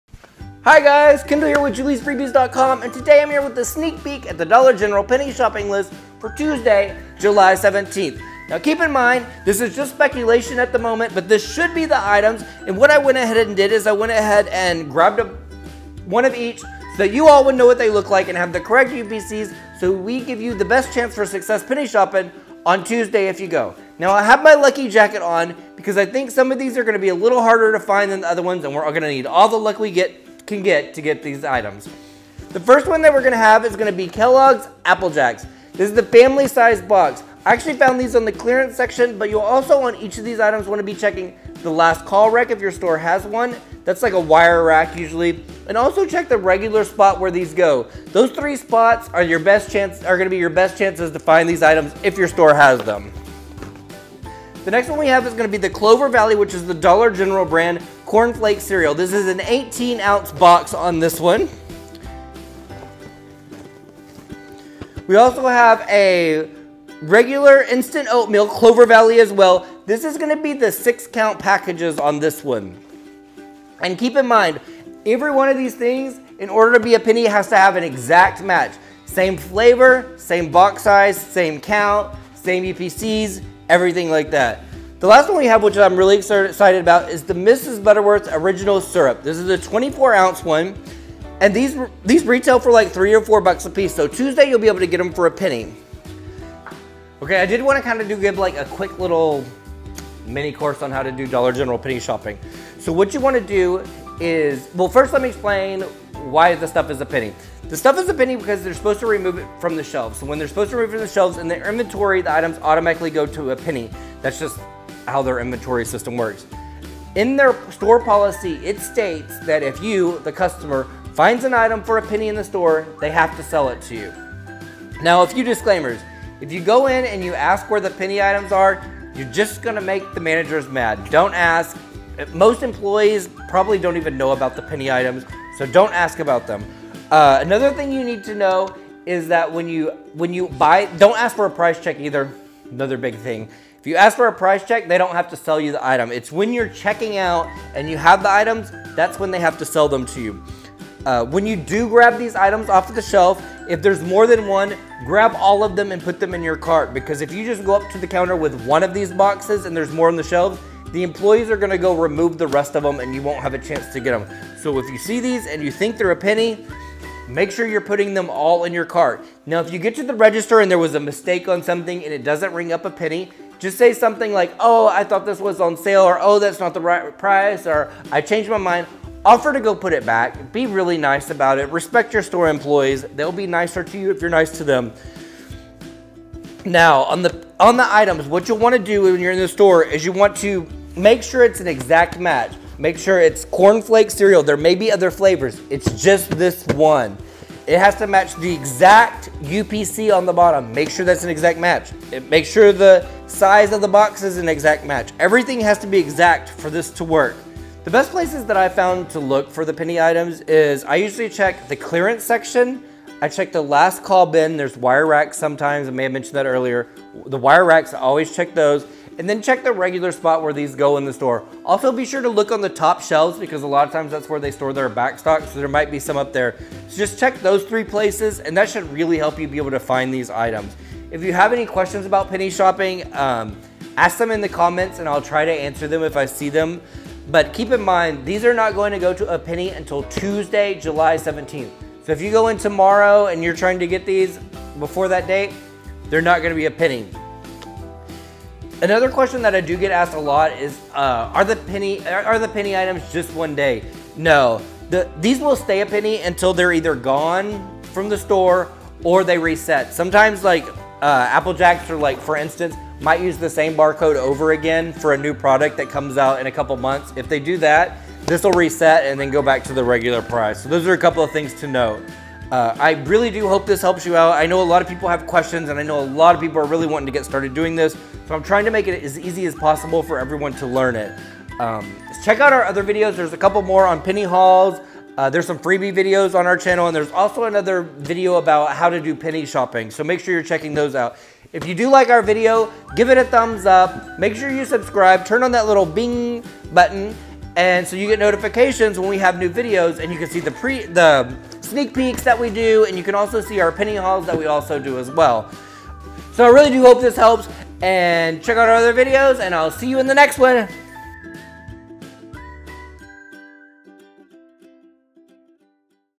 Dollar General Penny List - July 17, 2018
FIRST: The number 1 rule of penny shopping is not to discuss it with employees. Yes they know it exists, yes corporate knows, yes managers know, but asking where pennies are or fighting with them could get penny shopping discontinued.

HEADS UP! Looks like we have new pennies coming to Dollar General on July 17, 2018. I did some legwork for everyone and picked up the items on the list so you guys will know what they look like as well as the UPCs.

I also put in a very short how to at the end of this video, so if you are new to penny shopping, helpfully that will help out :)

Keep in mind that these are NOT official until we can verify on Tuesday, but generally the sneak peeks are pretty accurate.